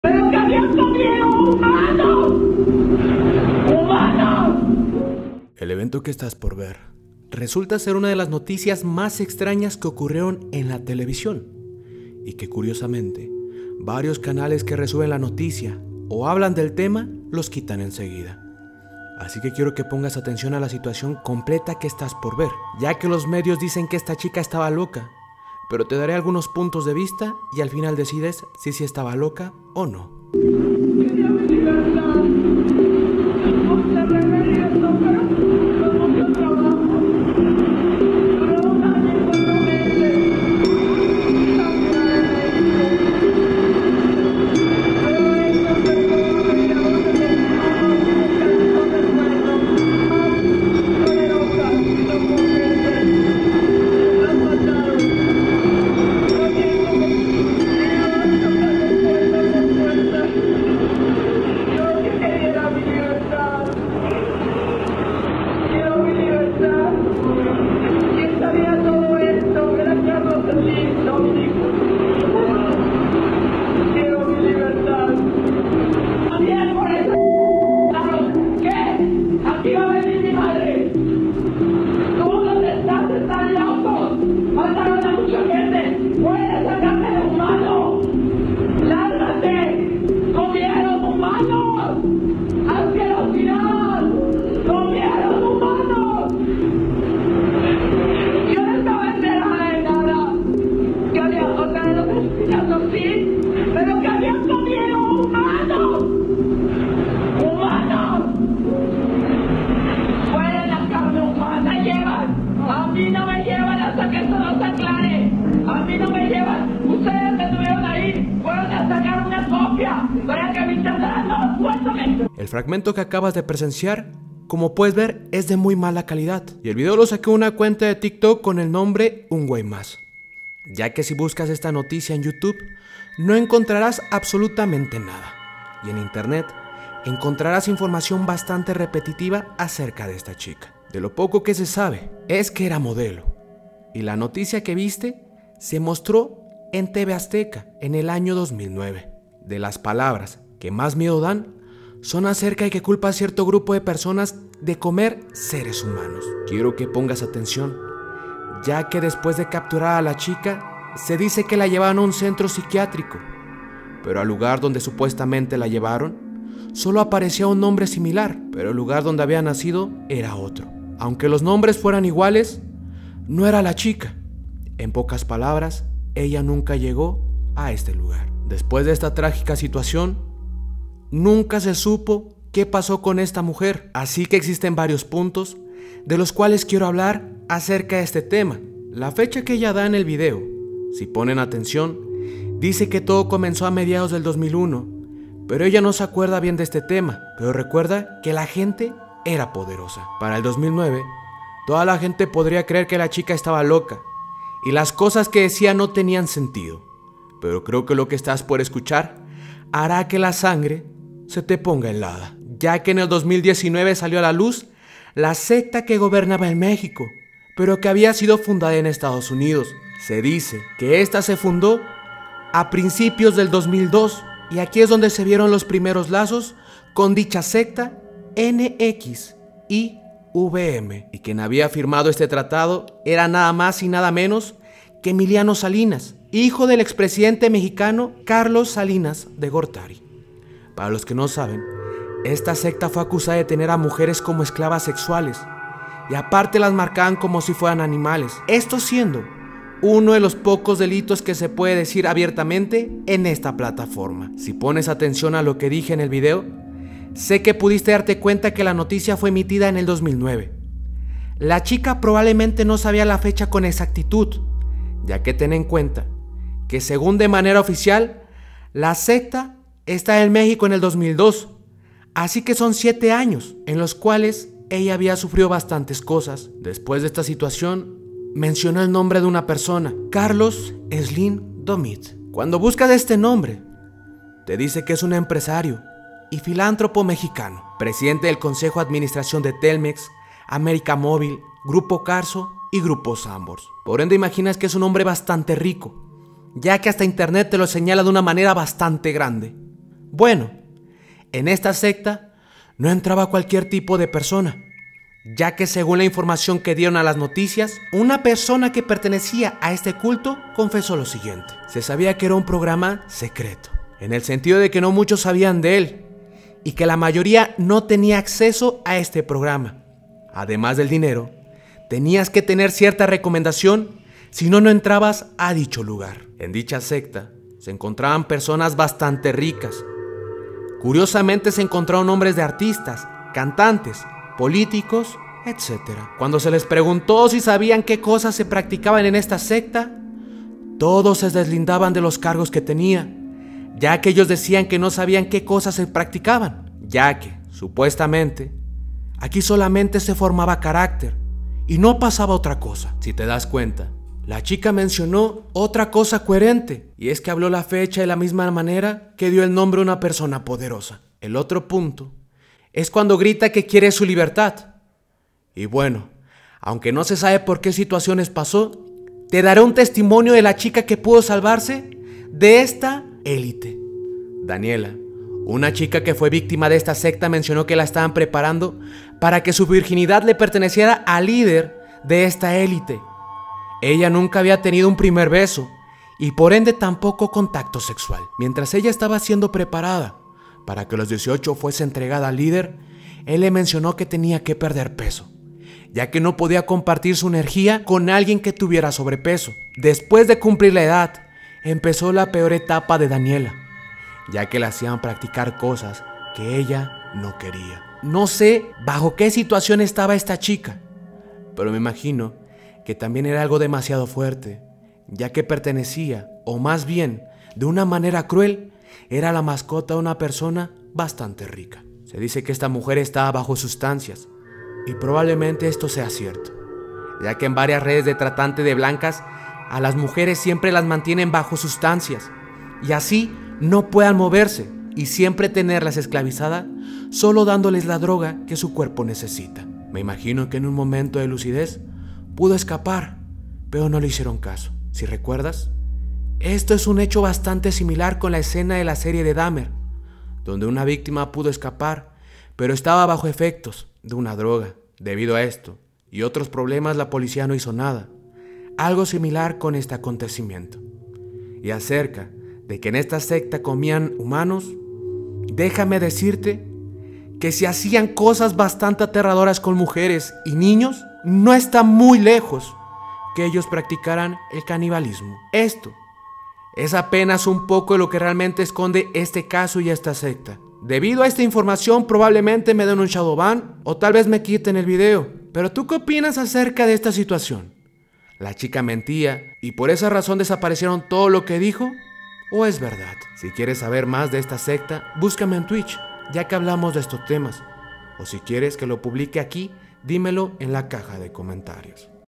“Habían comido humanos” EL CASO DE GABRIELA RICO
Hola mis amigos espero puedan apoyar este video, ya no tengo mas que decir no se si leen la descripción xd